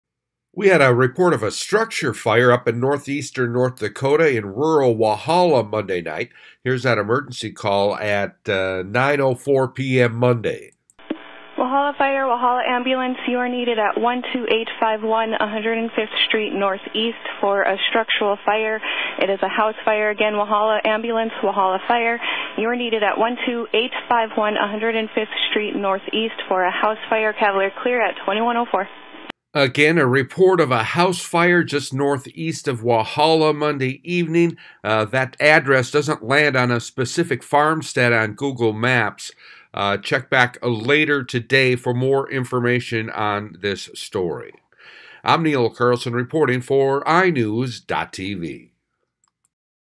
Report Of Monday Night House Fire Near Walhalla, ND
WALHALLA, ND (iNewZ.TV) Firefighters were called to a house fire northeast of Walhalla, North Dakota Monday night.
* Check back later for more details.